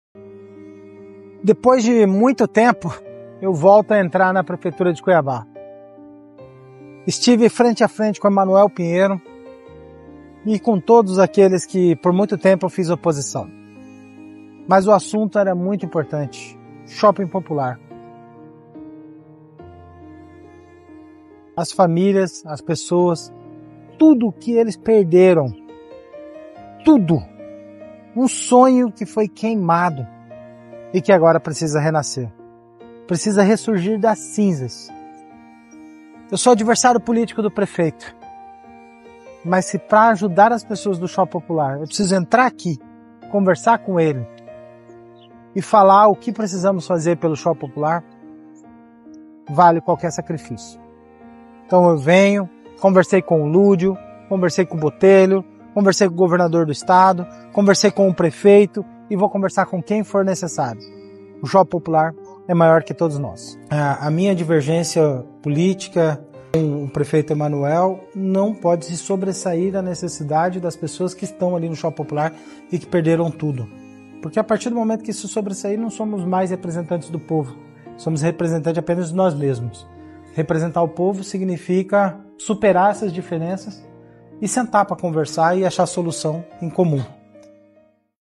É necessário falar com todos para ajudar o Shopping Popular de Cuiaba
O que você acha:
Devo ou não falar com Emanuel, Botelho, Lúdio e todos que precisarem para ajudar as pessoas?